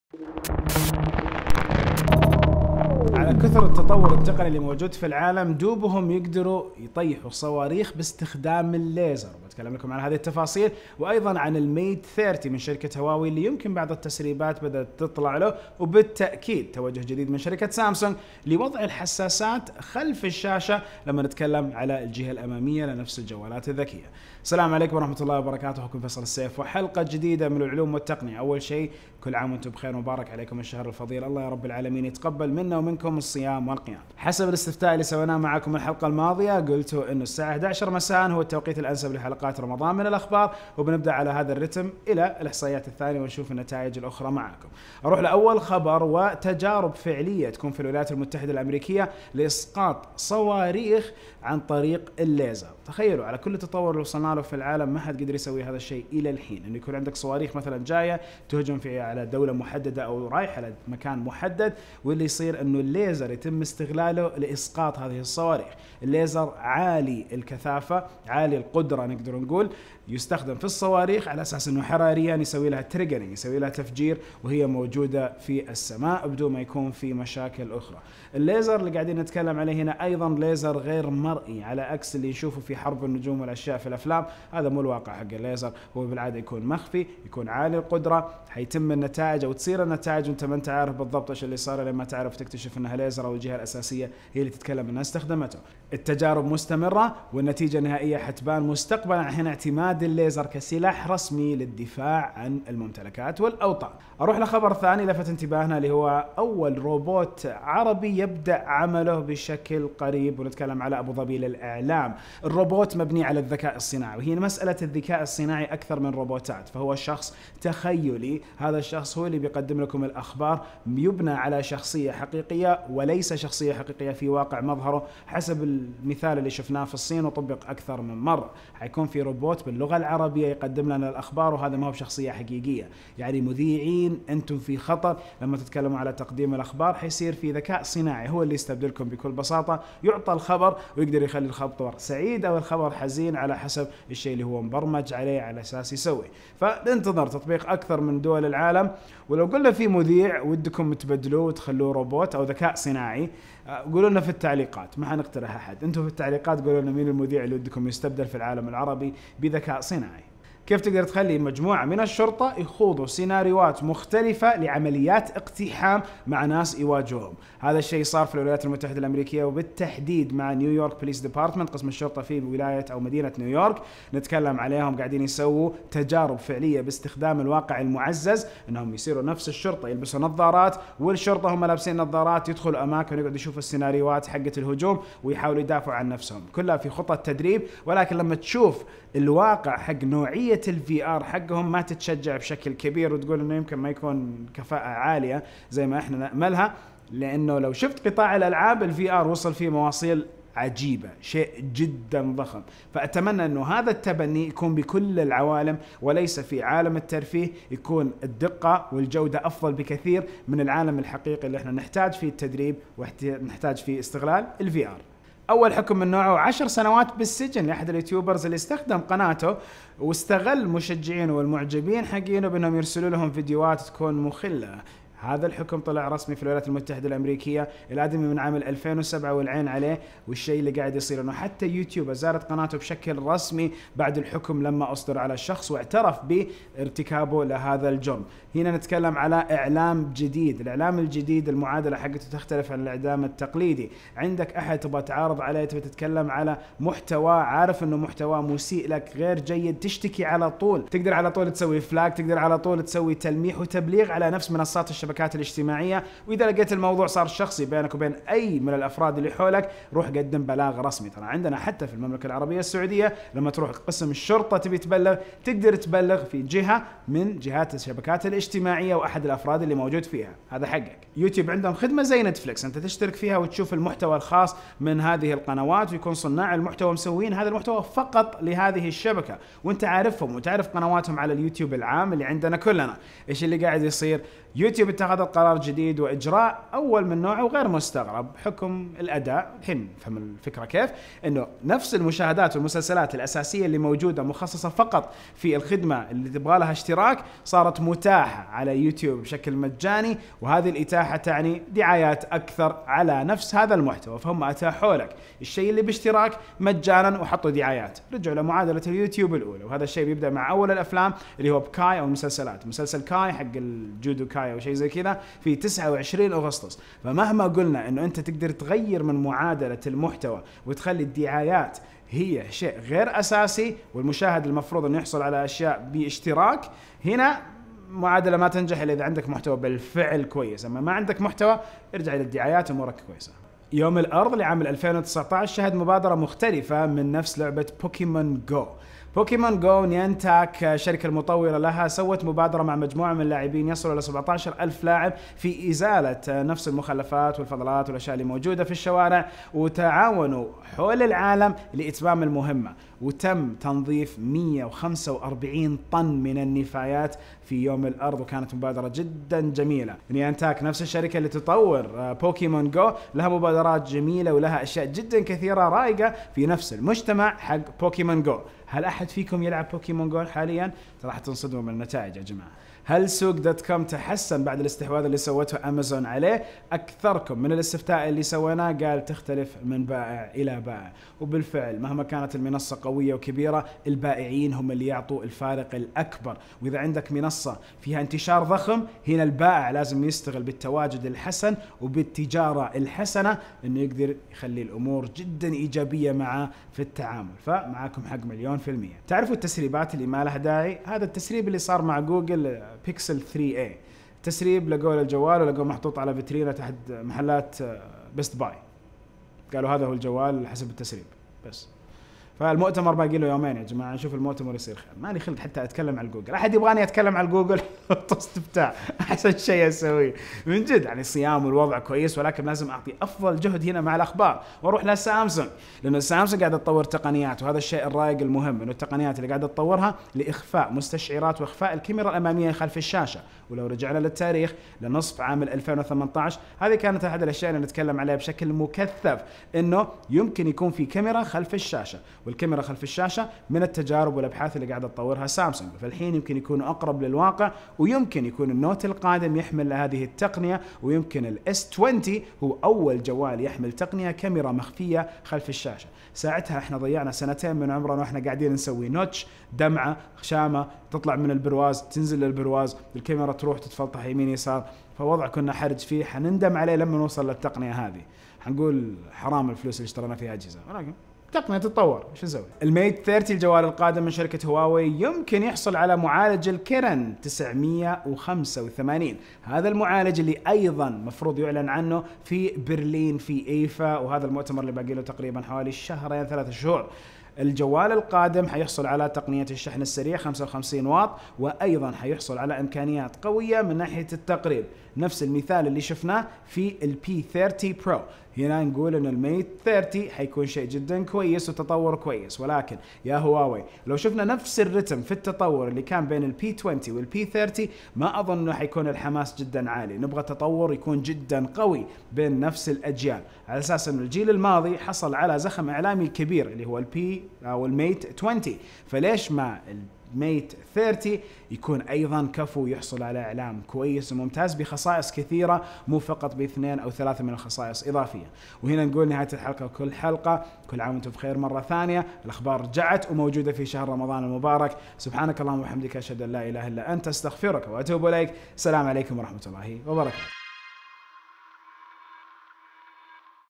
الليزر لاسقاط الصواريخ! الالعاب الالكترونية لتدريب الشرطة! بداية تسريبات الميت 30 برو!
00:46 تجارب اسقاط الصواريخ باستخدام الليزر
01:44 اول روبوت عربي يبأعمله كمذيع اخبار
02:40 شرطة نيويورك و تدريبات الـ VR
03:35 سجن يوتيوبر 10 سنوات
04:30 المحتوى الاصلي ليوتويب بيكون متوفر للجميع
05:33 بوكيمون ساهمت في تنظيف الارض
06:38 تسريب البيكسل القادم في احد المتاجر
07:04 سامسونج وكاميرا خلف الشاشة
07:52 بداية تسريبات الميت 30 برو

b
حساب فيصل سناب تشات:

حسابات انستجرام الخاصة بفريق لعمل: